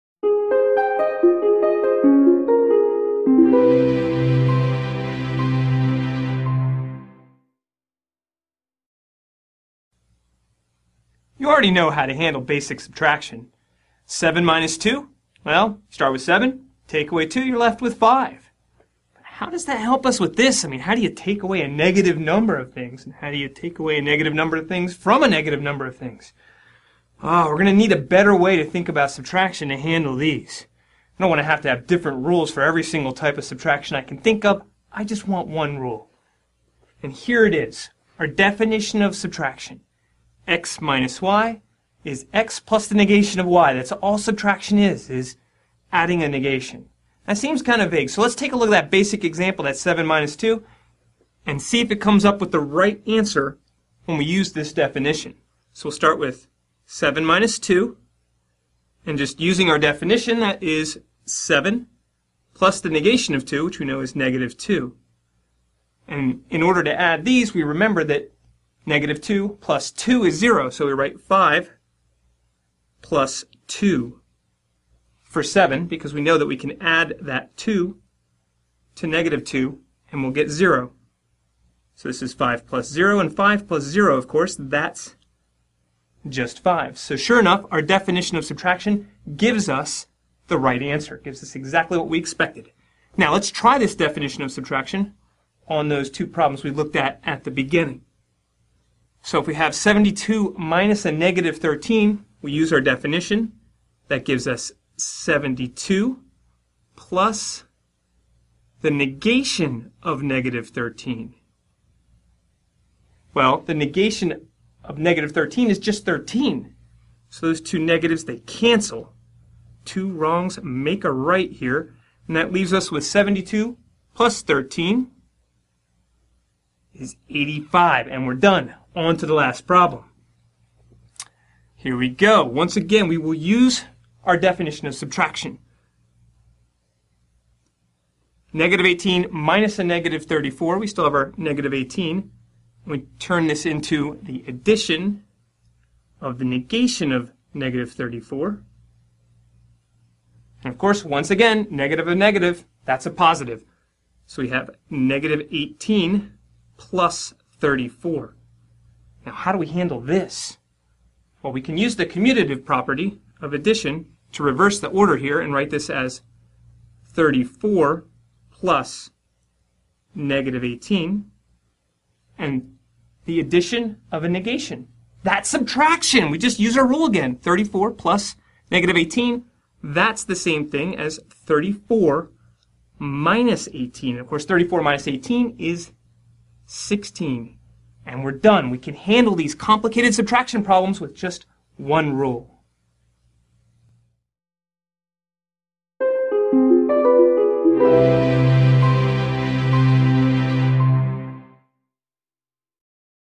Art of Problem Solving: Subtraction with Negatives Part 1
Art of Problem Solving's Richard Rusczyk introduces the definition of subtraction and subtraction with negative numbers.

This video is part of our AoPS Prealgebra curriculum. Take your math skills to the next level with our Prealgebra materials: